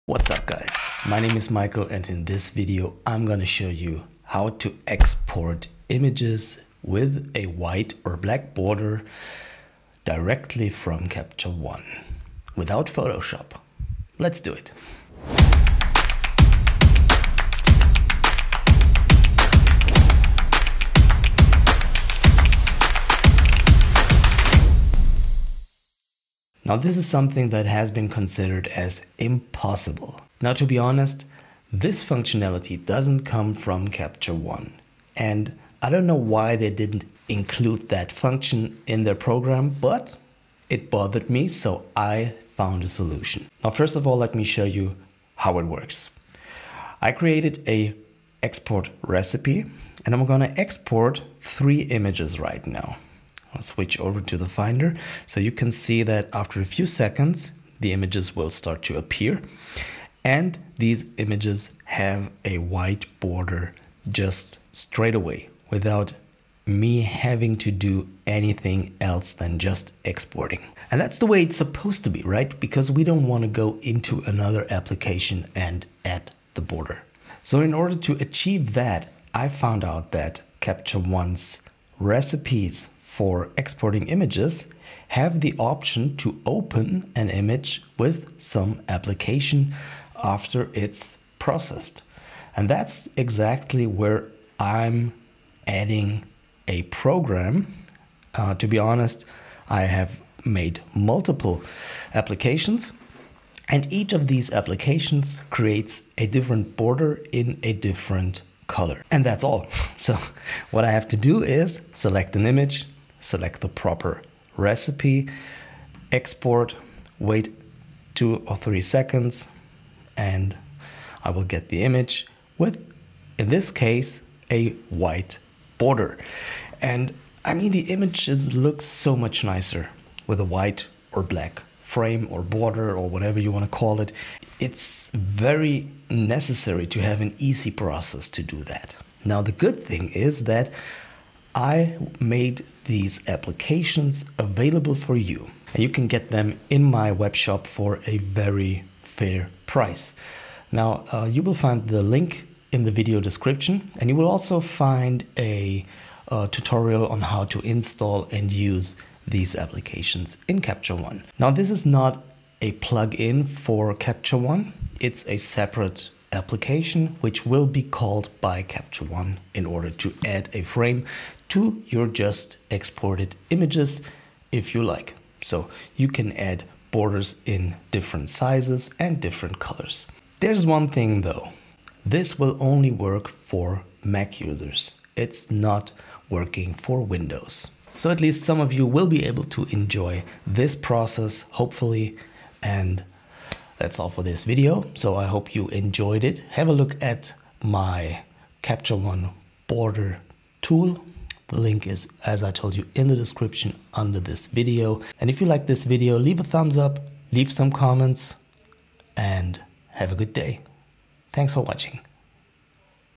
Add white border when exporting from Capture One Pro
We agree. Pictures with a plain border around them are just nicer. Not for nothing have images in every reasonable exhibition frames. Unfortunately, Capture One Pro lacks the ability to automatically add a border when exporting images. It is then quite laborious to add this frame with other programs. Maybe you’ve already asked google for all sorts of words to find out how to get a white frame around the images in Capture One Pro? Most people – including me just believe that they do not find the function – in reality it just does not exist.
Until now! I have developed a solution for it.

With my Border Tool for Mac OS and Capture One Pro, when exporting your images you can now directly and without further effort add an optional white or black border in 10px, 20px, 40px, 60px, 80px or 100px width. Of course, your picture will not be cropped, but expanded by the frame width.

Overview of the functions:
  - Add frame or border in Capture One
  - border width adjustable: 10px, 20px, 40px, 60px, 80px, 100px.
  - White Frame or Black Frame for Capture One Pro
  - Integration with Capture One Pro in processing presets
  - Exporting images with a border only takes about 1 second longer
  - usable directly from MacOS for already existing pictures
  - ONLY for MacOS! Not for Windows.
  - The surrounding frame is the same width everywhere
  - no polaroid effect